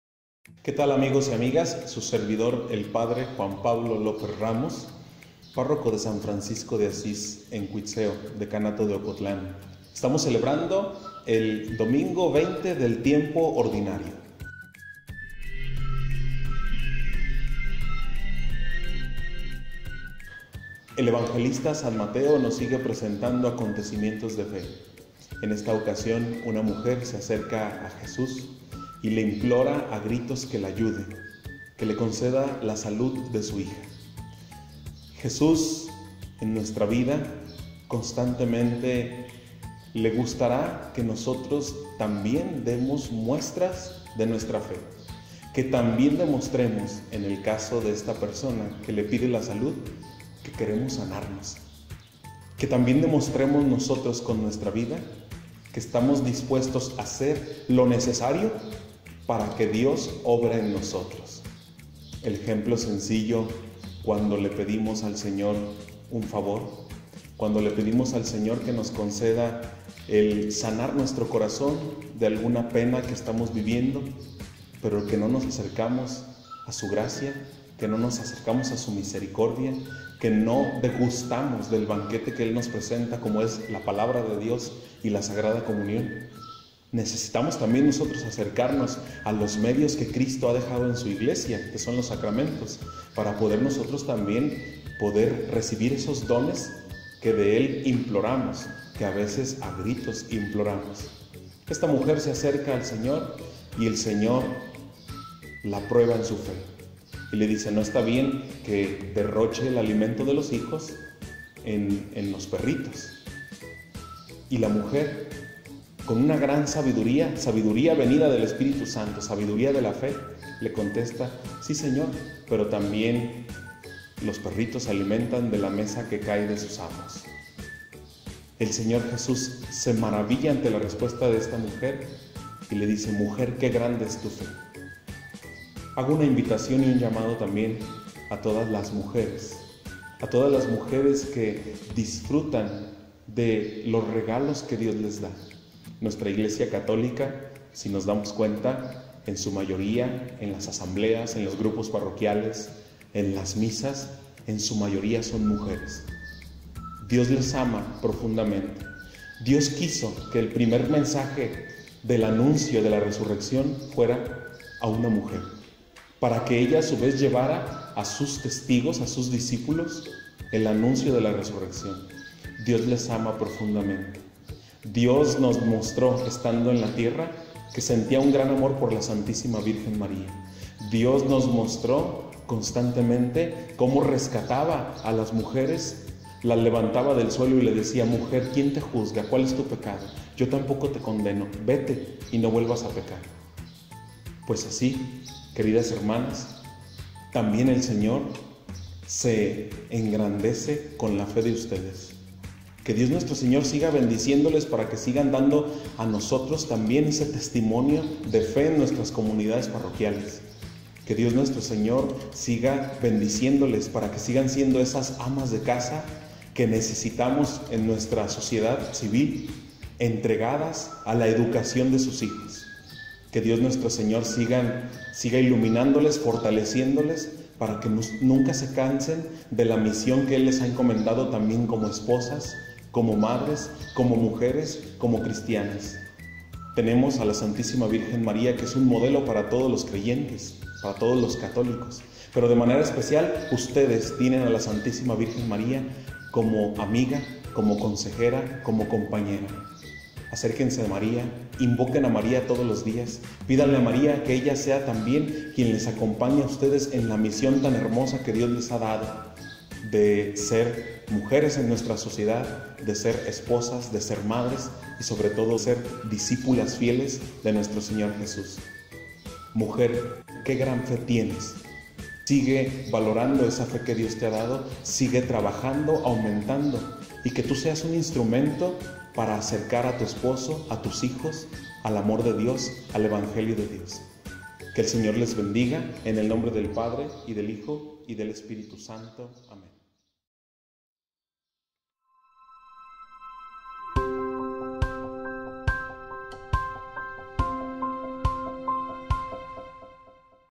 La Palabra del Domingo | 20 de Agosto 2017.
Te compartimos la reflexión de este Domingo 20 del tiempo Ordinario.